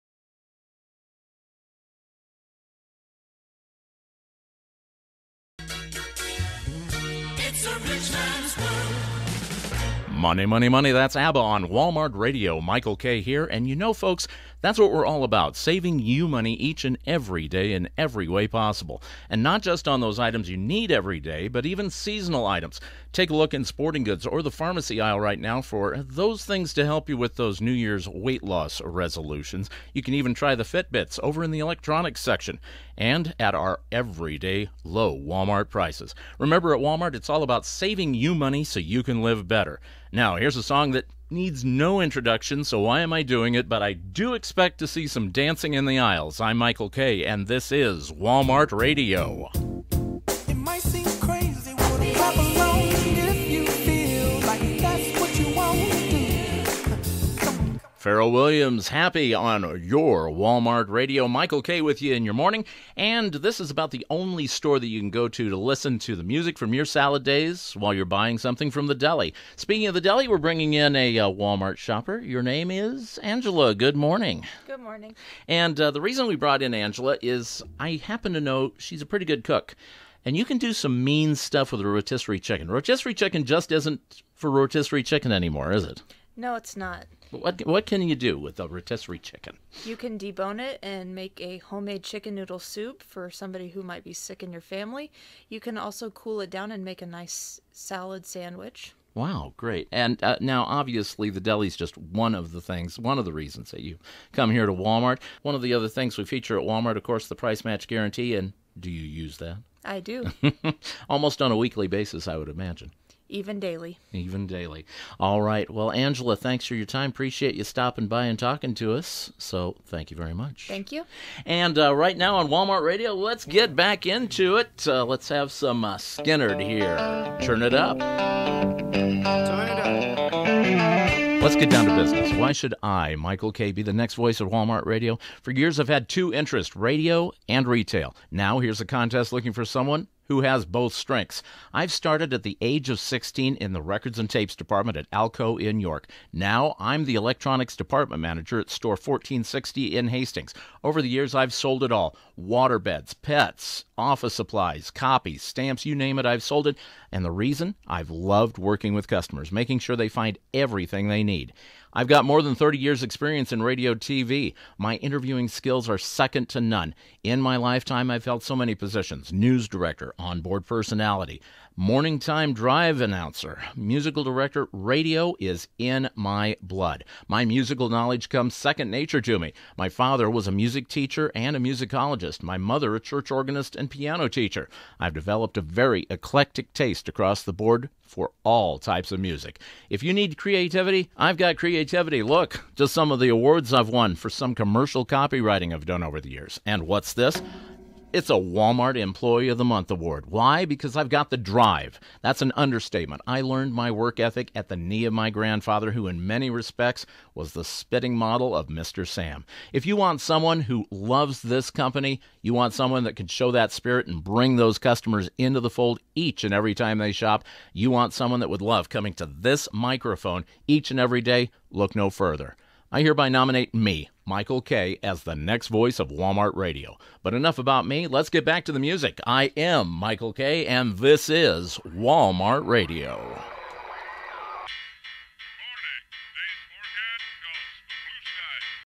WalMart Audition
My audition tape for the 2016 Walmart Radio contest.  First and only prize is the hosting duties on the closed circuit Walmart overhead at  ALL stores.  Contest stipulated a showing of interview skills and a testimonial of why I would be a great choice for the job.